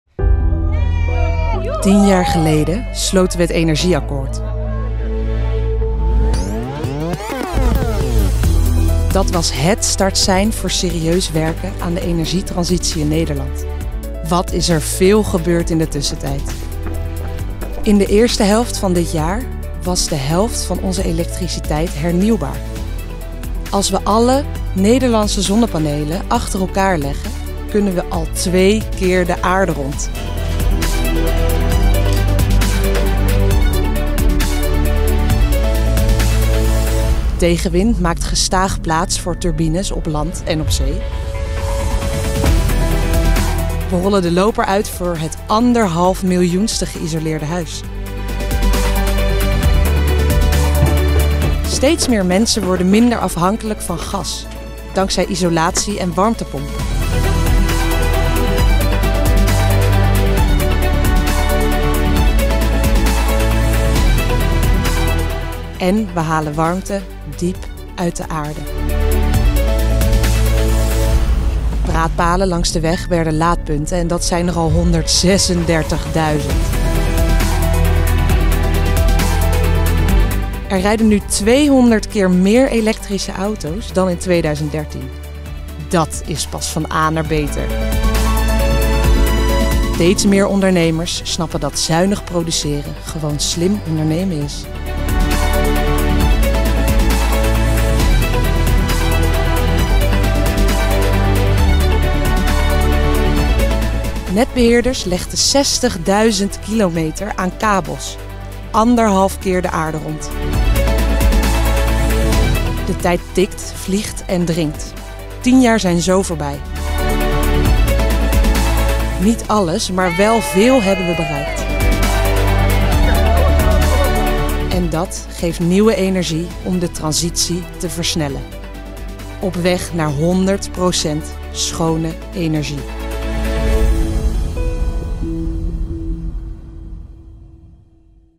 Tien jaar klimaatbeleid, sinds Energieakkoord: bijna vier keer zoveel duurzame energie
Het is vandaag tien jaar geleden dat het Energieakkoord getekend werd. Deze voorloper van het Klimaatakkoord markeert de start van serieus klimaat- en energiebeleid in Nederland. Er is sindsdien veel bereikt, en er is ook nog veel te doen. In tien jaar steeg het aandeel duurzame energie van 4,7 procent in 2013 naar 16,7 procent nu.

“Als je nog veel te doen hebt, vergeet je soms hoeveel je hebt bereikt. Met ambitie, daadkracht en samenwerking van vele partijen heeft het energieakkoord de energietransitie onomkeerbaar versneld. Hopelijk geeft dat de moed en het vertrouwen om nog sneller door te gaan en de vele uitdagingen steeds weer te overwinnen!” zegt Olof van der Gaag, voorzitter Nederlandse Vereniging Duurzame Energie (NVDE). De NVDE maakte dit filmpje om dit te vieren. Daarin komen deze cijfers voor:

Hernieuwbare elektriciteit:
2013:  5%
2023: 46%

Zonnepanelen:
2013:   2 miljoen
2023: 54 miljoen, Alle Nederlandse zonnepanelen achter elkaar gelegd, zouden twee keer de aarde rond gaan.

Windturbines op zee:
2013:   96
2023: 671, Inmiddels subsidievrij!

Goed geïsoleerde huizen:
2014:    123.000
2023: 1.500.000

Warmtepompen in woningen:
2012:   70.000
2023: 500.000

Aardwarmtebronnen:
2013:   8
2023: 36

Laadpunten:
2013:      6.000
2023: 136.000

Elektrische auto’s op de weg:
2013:      1900
2023: 387.000

Zware industrie bespaarde 2,5 megaton CO2 per jaar: evenveel als de jaarlijkse uitstoot van een kolencentrale.
60.000 km nieuwe elektriciteitskabels sinds 2013.